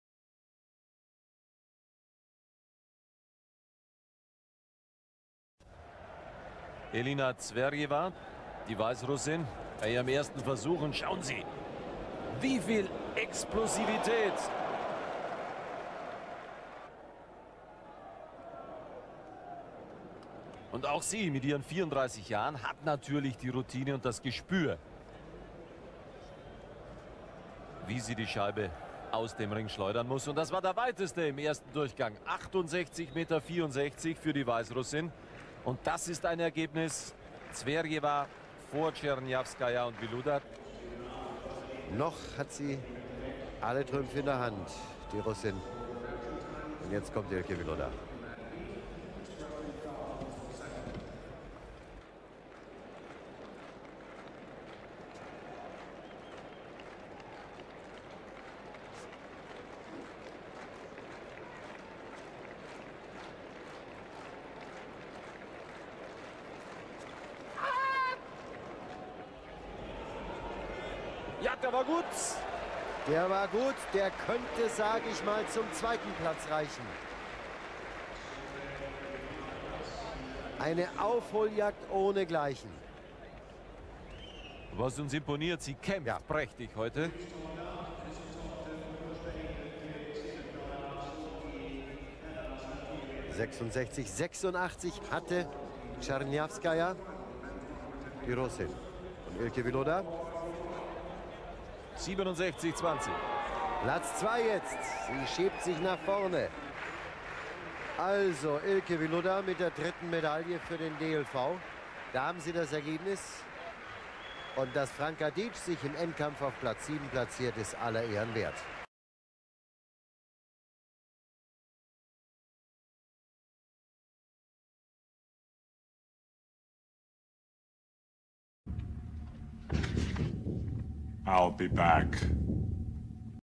Highlights DISCUS WOMEN 1995 WORLD CHAMPIONSHIPS.
1  Ellina Zvereva (BLR) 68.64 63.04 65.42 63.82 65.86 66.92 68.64 m
2  Ilke Wyludda (GER) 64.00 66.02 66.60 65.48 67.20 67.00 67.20 m